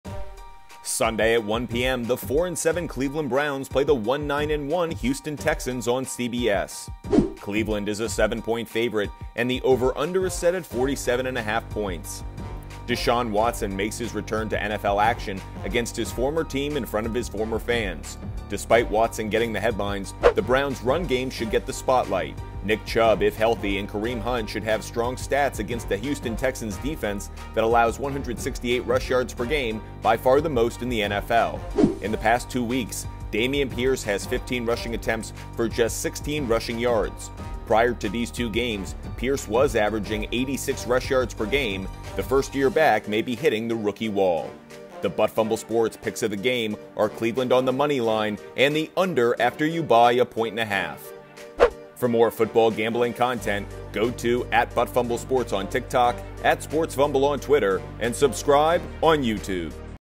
NFL Week 13 Game Preview and Pick: Browns at Texans
How is Butt Fumble Sports betting Browns at Texans?

Here are the gambling trends, stats, storylines, and game pick you need before kickoff!